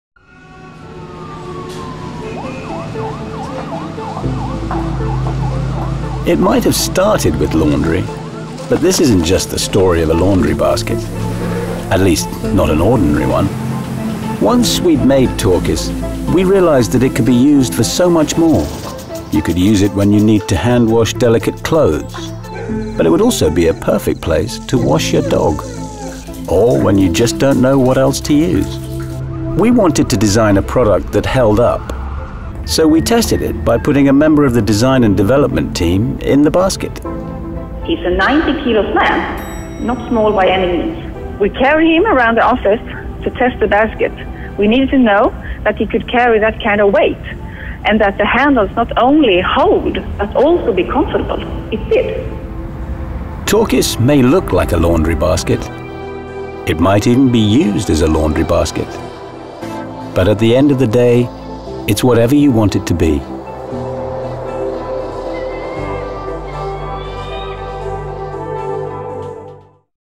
TORKIS - so much more than just a laundry basket
This is the story of TORKIS laundry basket. But it's also a story of how we, even for the most basic products, go through some unconventional testing. Turns out, TORKIS can be used for a lot more than just your dirty laundry.